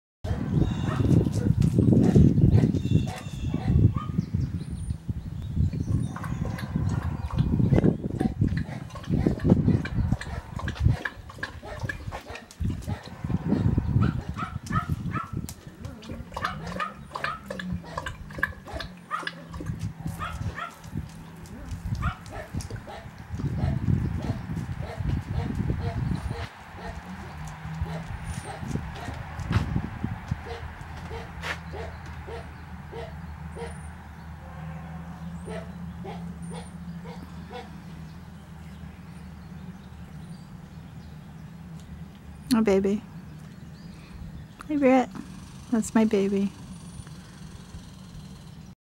Britney hip leg problem 04 17 15
This is Britney, she's almost 12 now and has been the picture of health.  She survived parvo as a young pup, was adopted to a great family and then had to be returned to us due to a family health emergency six months later.   She never really got interest from other families as she was kind of "junkyard dogish", so she has spent her life here with us.   She actually saved me from a vicious attack from a coy-dog back in 2006 and was injured and had to have 180 stitches from the fight.  Not only has she been one of the greatest dogs I have ever known in my lifetime,  I owe it to her to do what I can for her.   She started with some gastro issues a couple months ago - stool has been loose and very smelly, and three days ago, she started with the hip/leg issue.  I do not know if they are related or not, but something is wrong with my girl.   We have a vet appt. on Tuesday the 21st of April, where she will get some diagnostic testing.   We have enough money raised to get her to the vet, and have blood work and fecal exam.   However, I'm sure he will want to do xrays of her hips, but we will not be able to do that at this time.